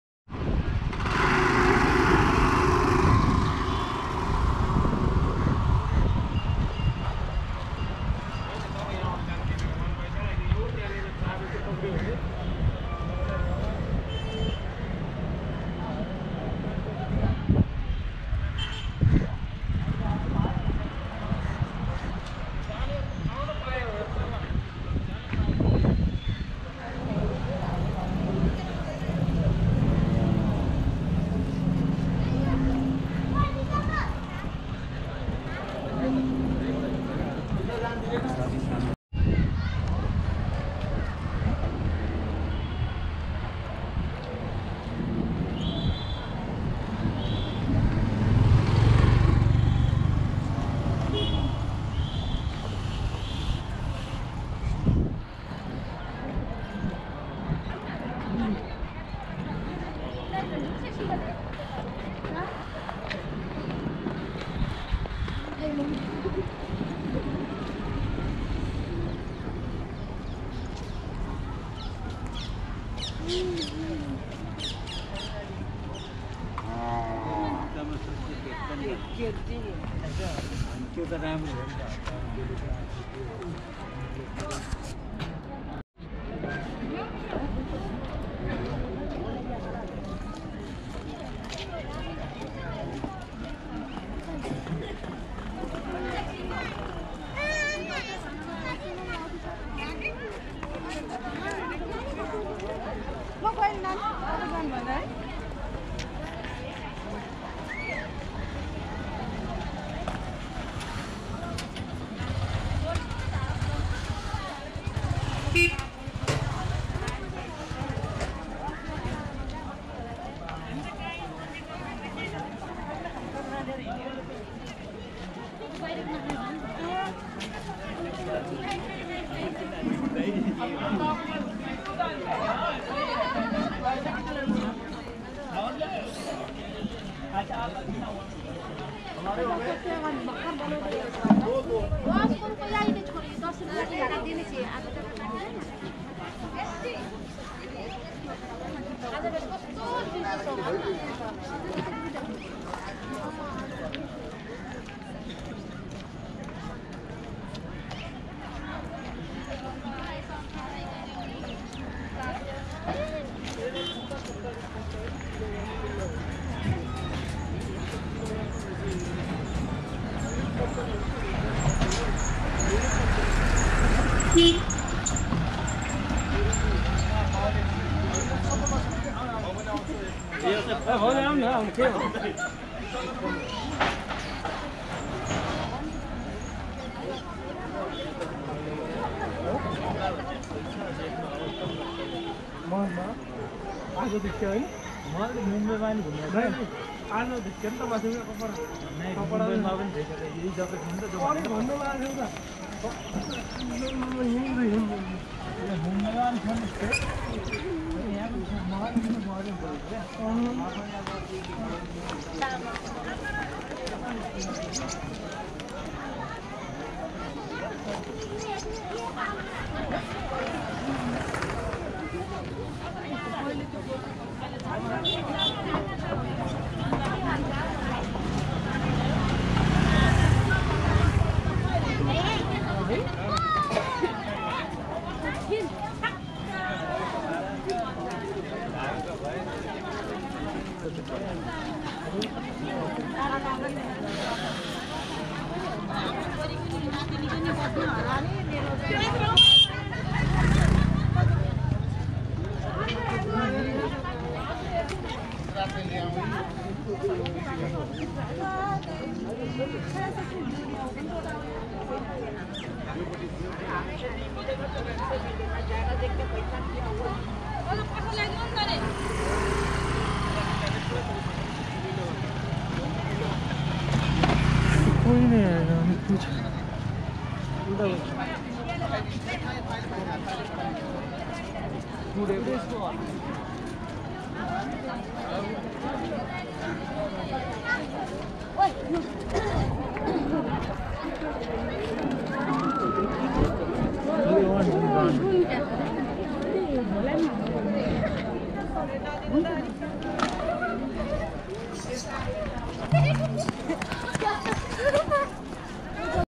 SHRI PASHUPATINATH TEMPLE | PASHUPATINATH ROAD | KATHMANDU | NEPAL
Shri Pashupatinath Temple Complex, went there on the 1st of February 2k20, to watch the Maha Shivaratri Pashupatinath Bagmati Aarati.